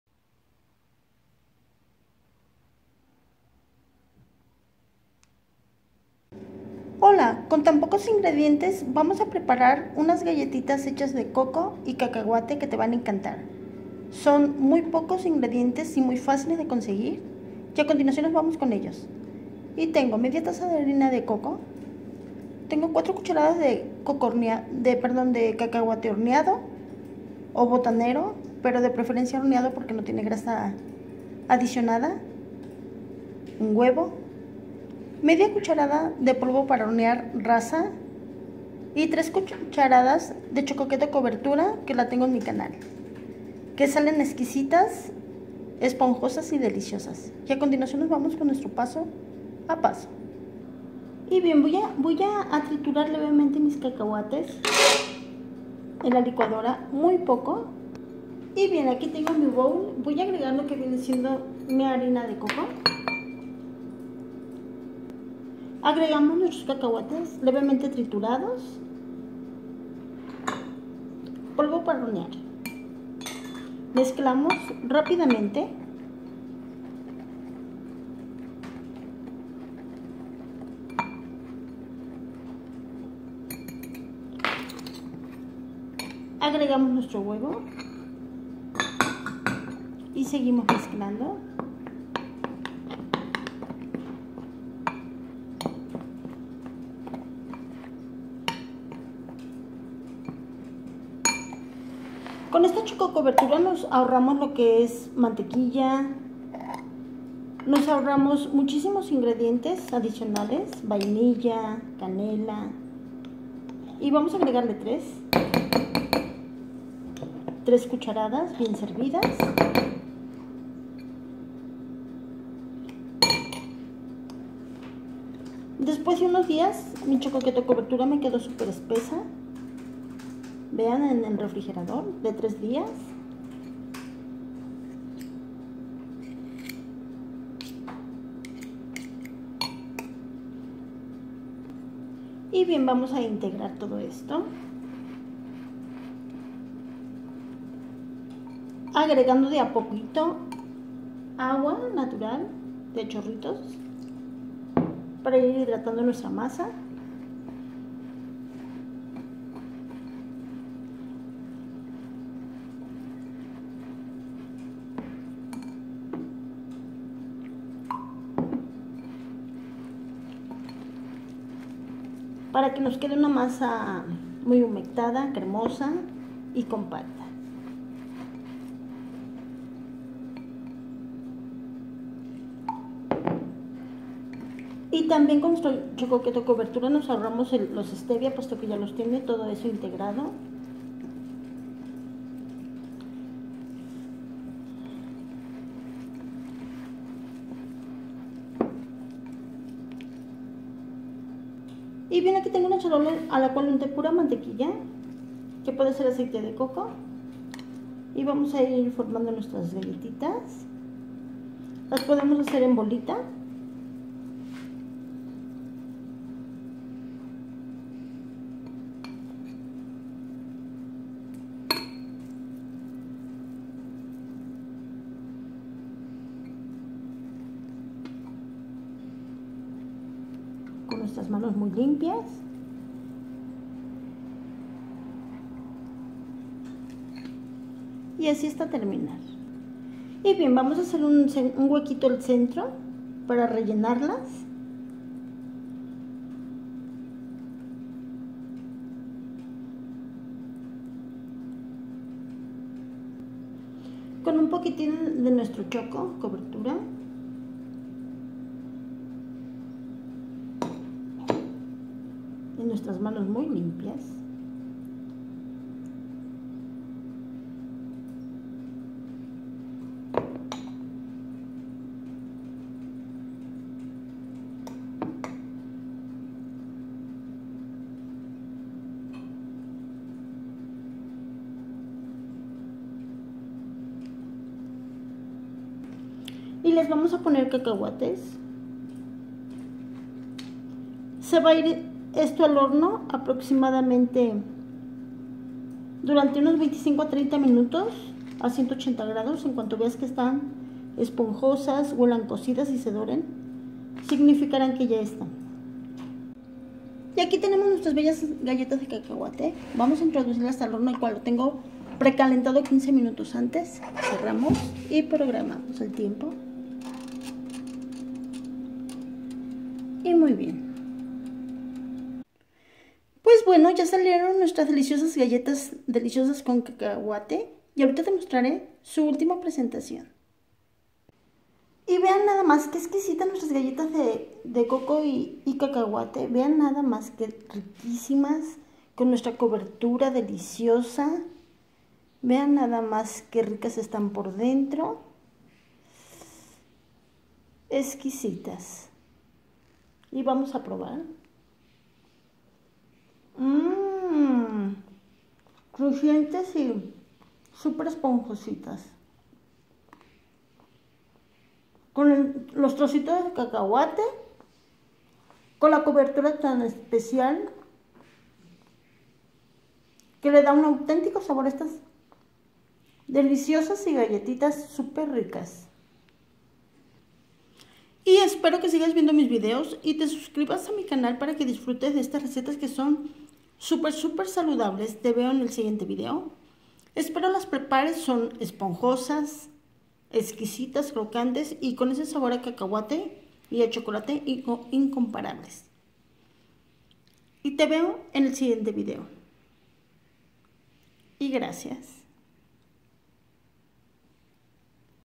GALLETAS🍘DE COCO🌻Y CACAHUATE SIN GLUTEN!
son unas galletas muy ricas esponjosas con el aroma del cacahuate y el chocolate🌰que las hacen mega deliciosas y muy fáciles de preparar🍘🌻🍘🌻🍘🌻🍘

INGREDIENTES
1/2 taza de harina de coco
4 cuch. de cacahuate horneado

INGREDIENTES
1/2 taza de harina de coco
4 cuch. De cacahuate horneado
1 huevo
1/2 cuch. cafetera rasa de polvo para hornear
3 cuch. de choco keto cobertura

para estar juntos en estas resetas super super saludables!🌻